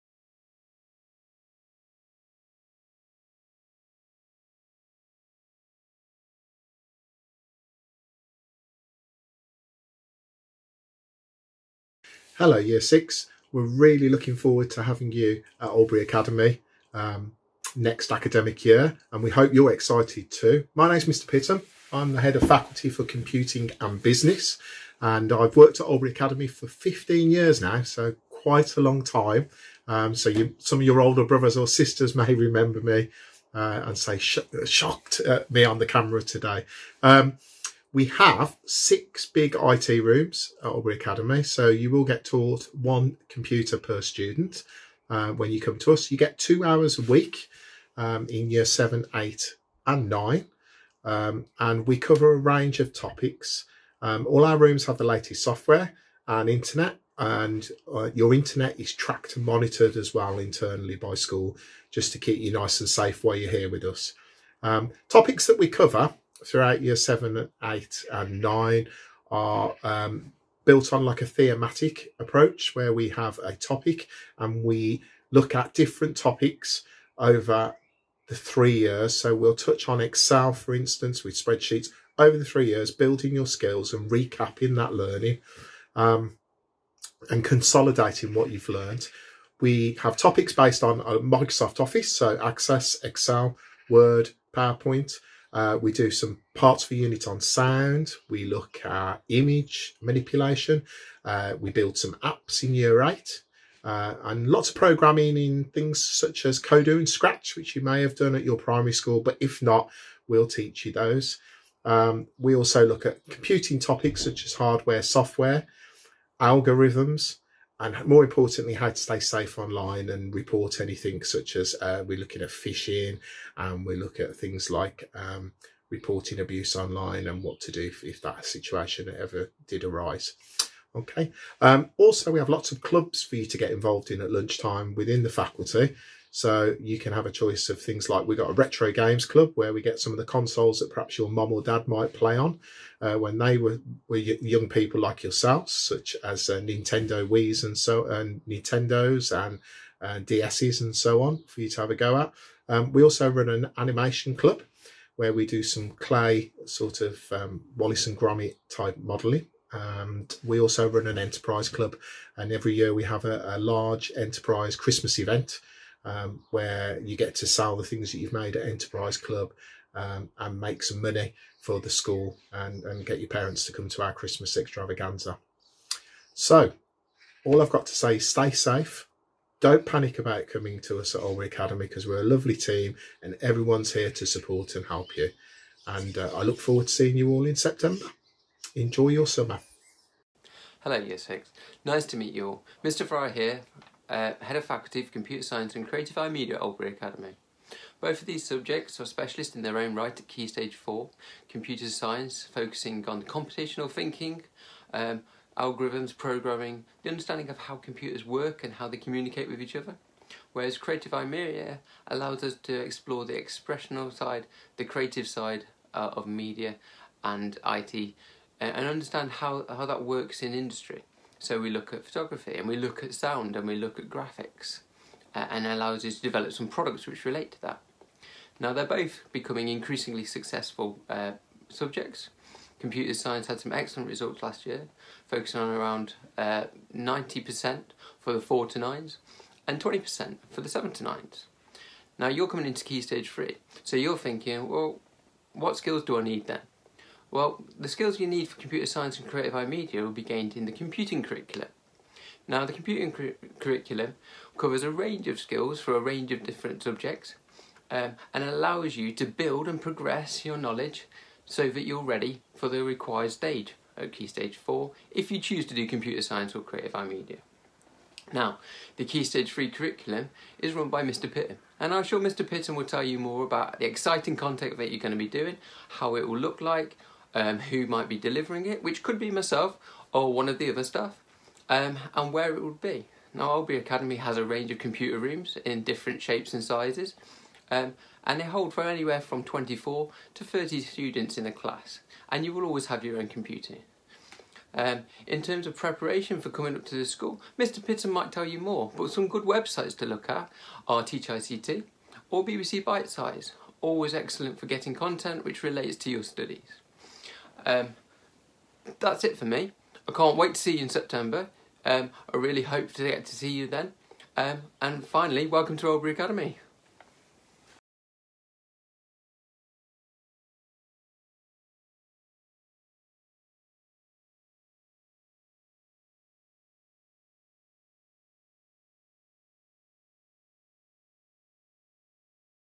Transition 2020 - Introduction to The ICT & Computing Department
As part of our Virtual Transition series this video gives you a brief introduction to ICT & Computing in Year 7 at Oldbury Academy.